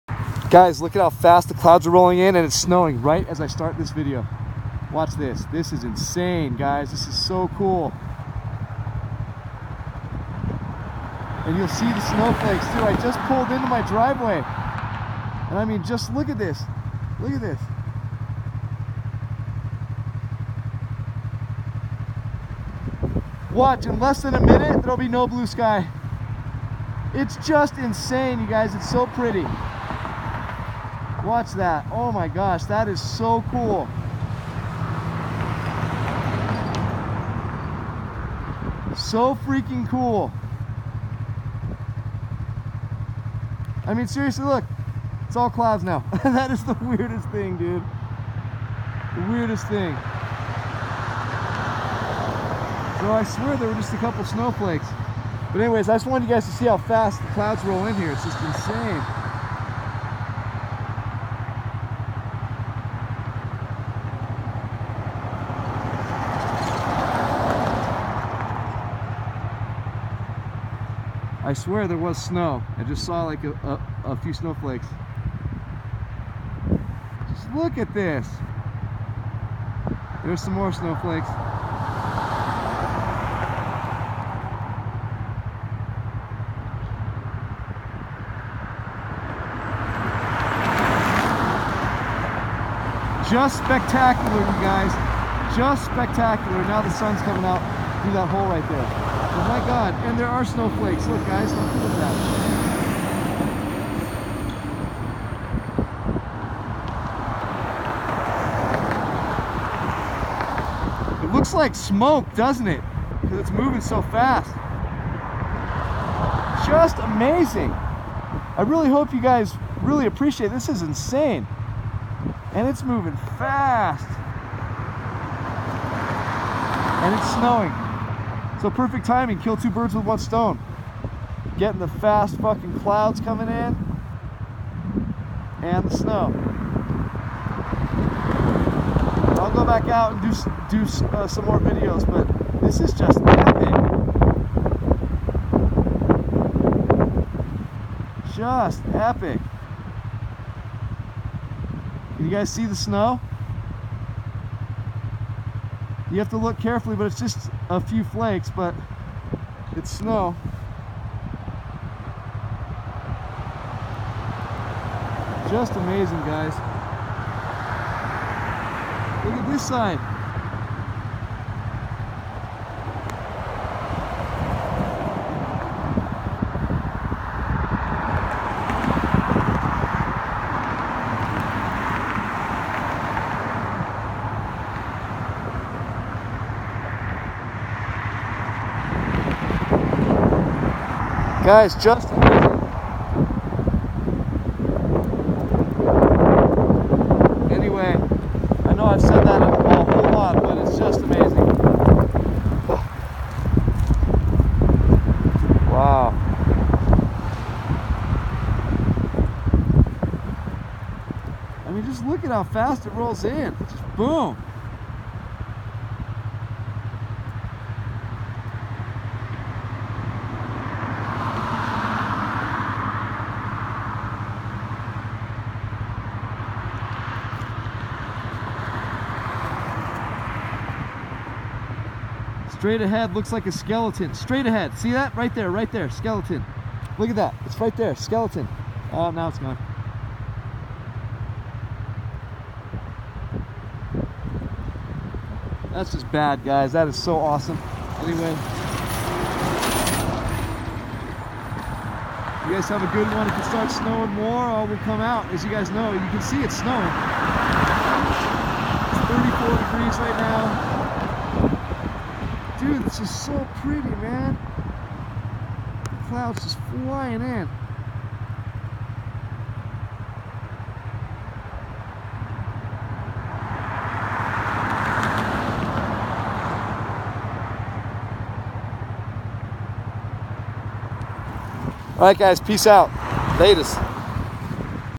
Big Bear Lake, CA. 2/12/2018. Storm clouds blowing in fast. So Magnificent. Majestic.
The clouds come in so fast it seems. Its cause we are closer to them but its still amazing to me always. They are moving so fast. Take care and enjoy.